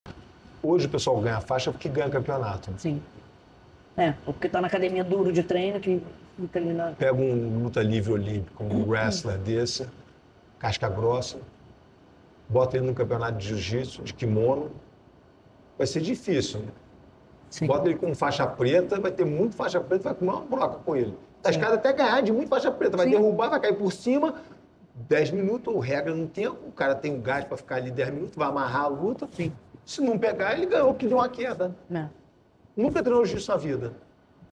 Faixa preta não deve ser apenas uma conquista de competição
Você concorda com o @realroyce?

@realroyce acredita que a faixa preta não deve ser apenas uma conquista de competição, mas o que é da arte marcial.

Suas palavras desafiam convenções e fazem todos nós repensarmos o significado da faixa preta atualmente.

Convidada: 🏆Royce Gracie - @realroyce
Host: 🎙 Kyra Gracie - @kyragracie
Produzido por: Agência Alastro - @agenciaalastro